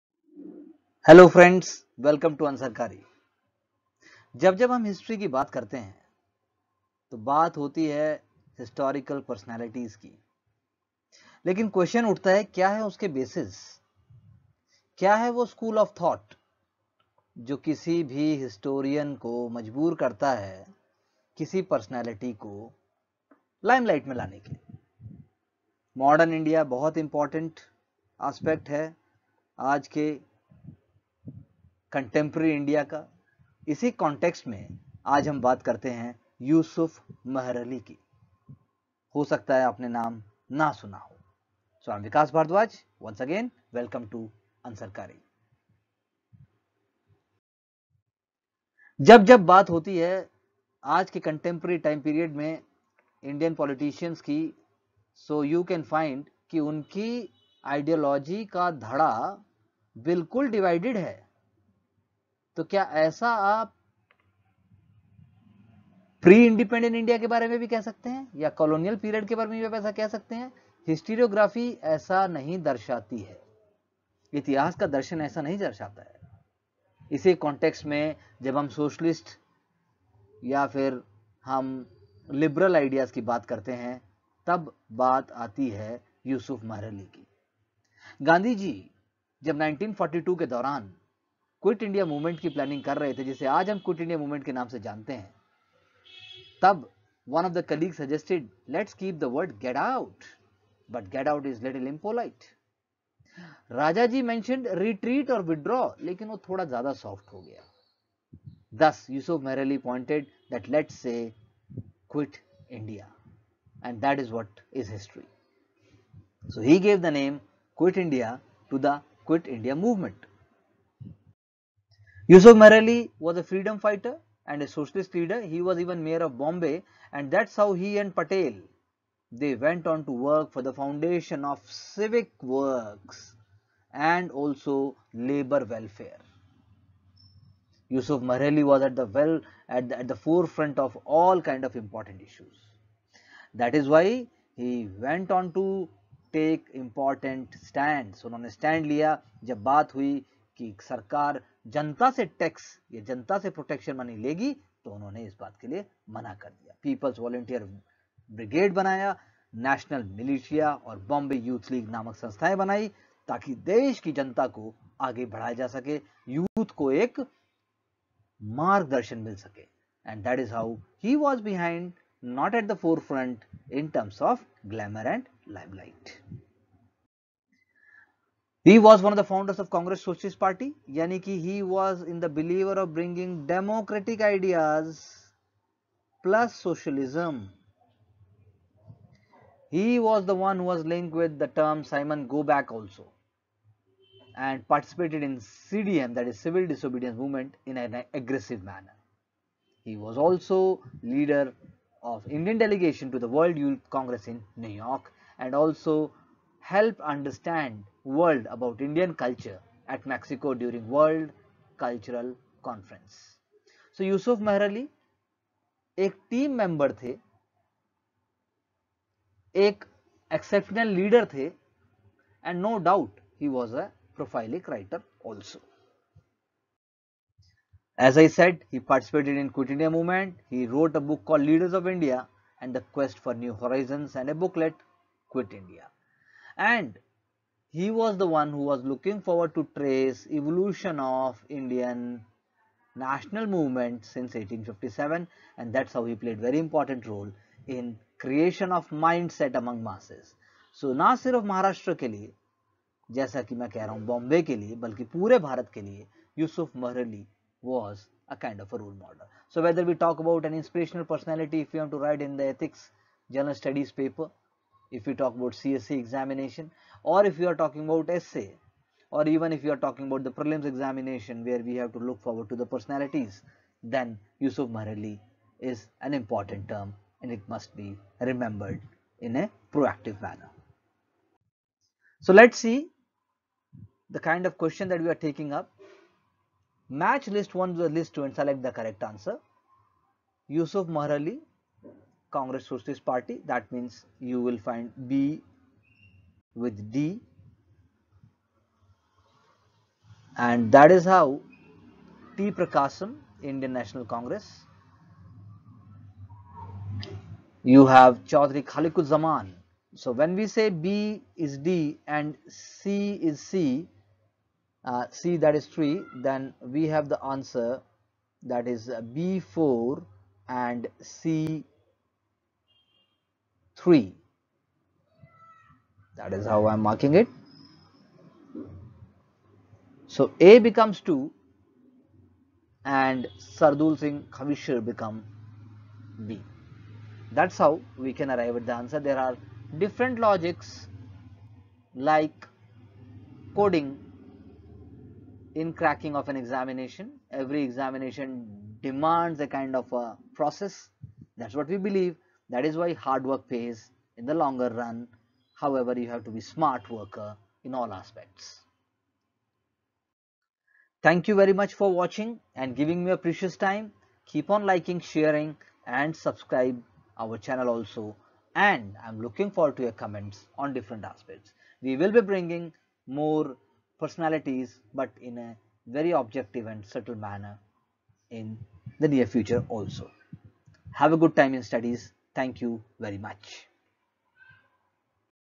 Yusuf Meherally
The video describes the contribution of Yusuf Meherally - the man who gave the memorable phrases like 'Simon Go Back' and 'Quit India'.